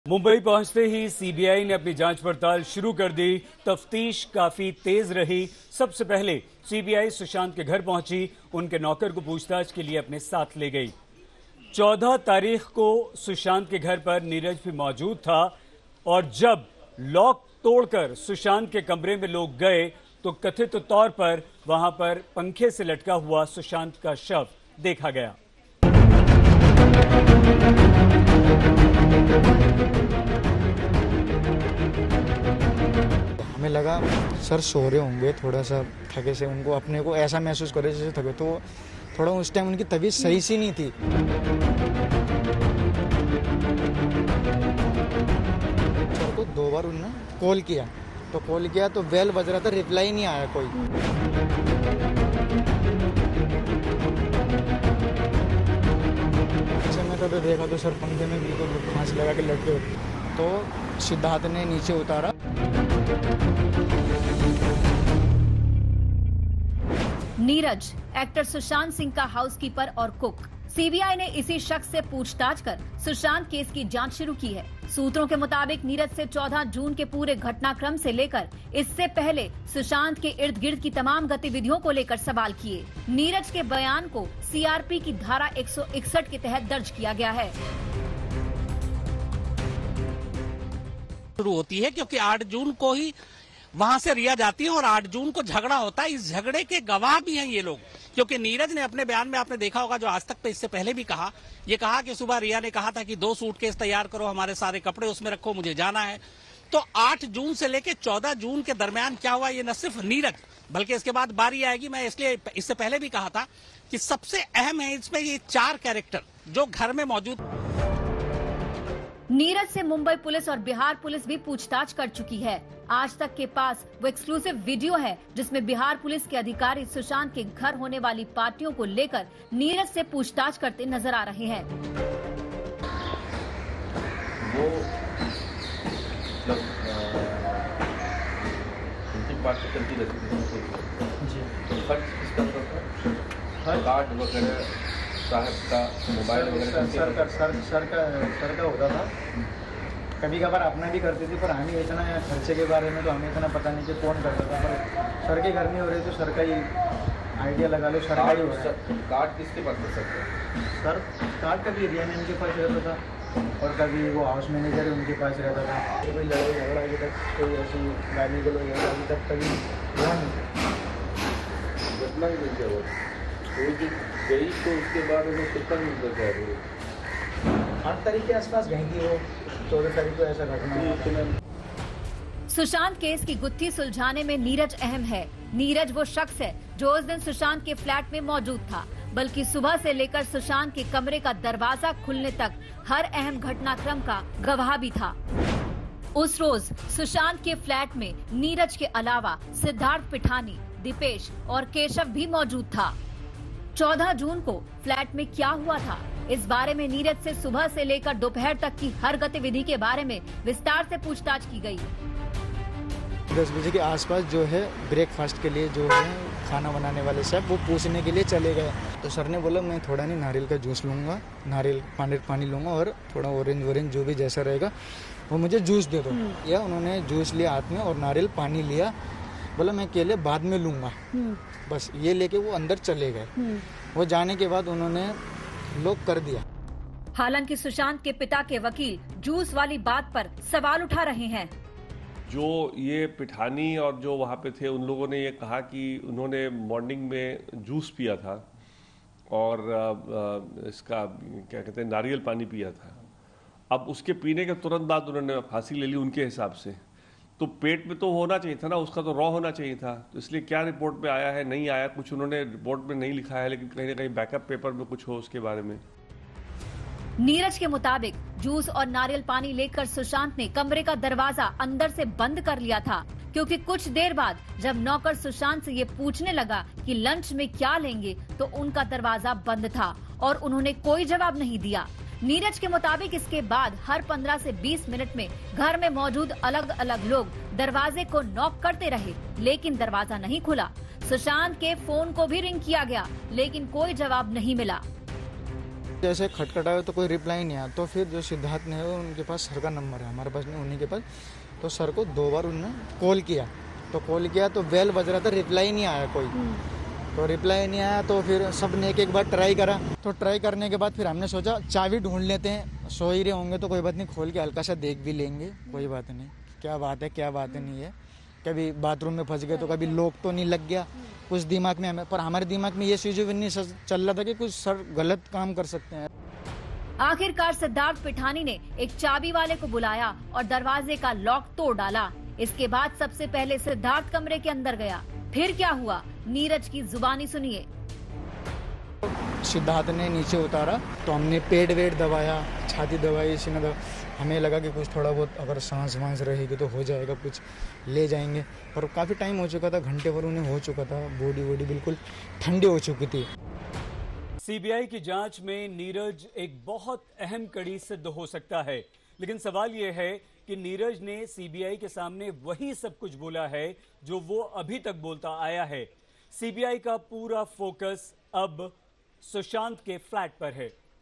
Sushant Singh Rajput की बॉडी को फंदे से किसने उतारा? कुक नीरज से 3 घंटे में CBI ने पूछे ये सवाल
मुंबई पहुंची सीबीआई टीम ने बिना मौका गंवाए तफ्तीश तेज कर दी. सबसे पहले अधिकारी सुशांत के हाउस कीपर को पूछताछ के लिए ले गए. नीरज भी 14 जून को सुशांत के फ्लैट में मौजूद था. जब लॉक तोड़कर सुशांत के कमरे का दरवाजा खोला गया और कमरे के अंदर कथित तौर पर पंखे से लटका सुशांत का शव मिला तो नीरज भी मौके पर था.